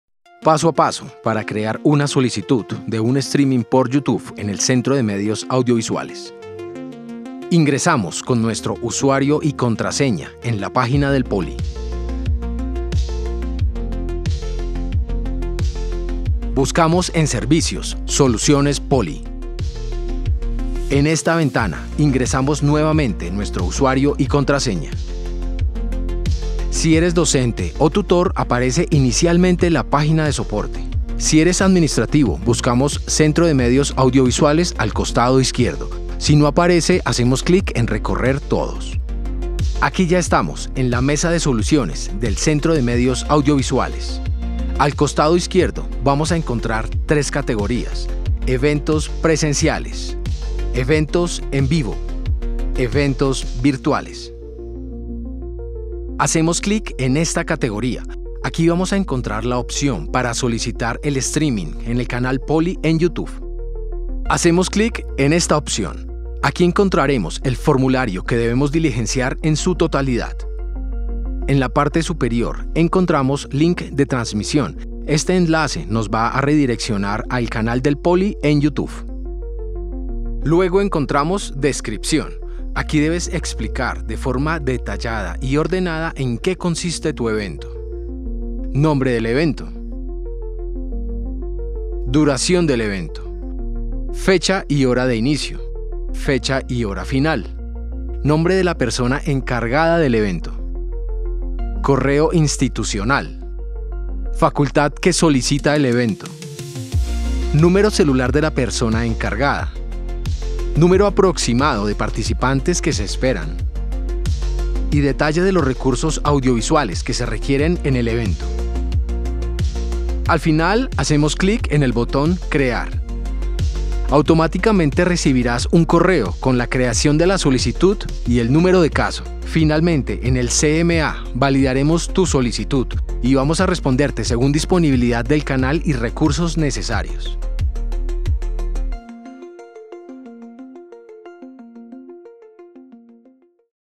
Crear una solicitud Streaming por Youtube
En este video aprenderemos a hacer DIRECTOS o transmisiones en vivo PASO a  PASO en Youtube. Descubre el método más fácil para transmitir en vivo en YouTube con nuestro tutorial paso a paso. Ideal para principiantes y expertos que buscan simplificar su flujo de trabajo.